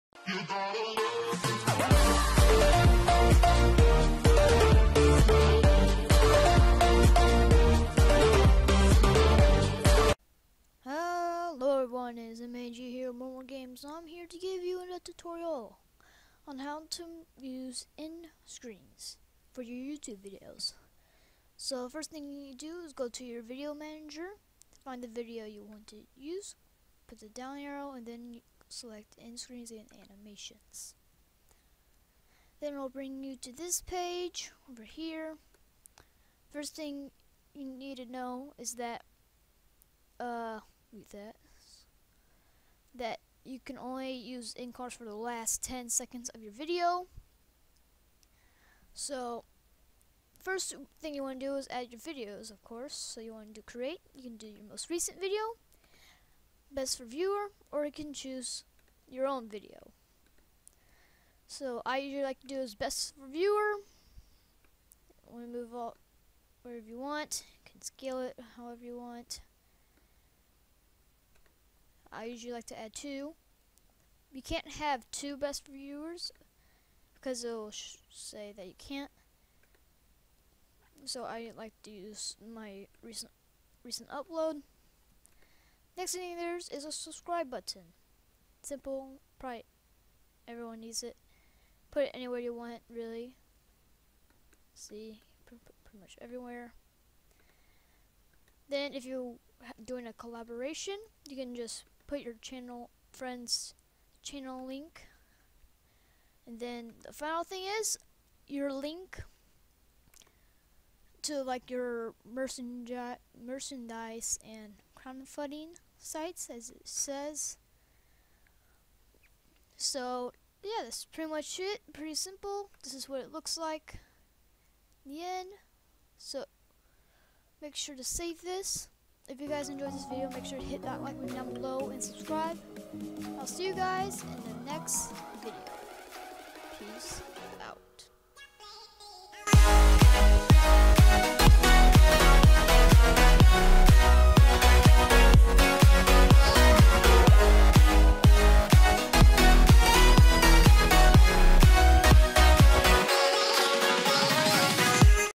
HOW TO USE END SCREENS
Today, I Made A Tutorial On How To Use End Screens For Your Youtube Videos.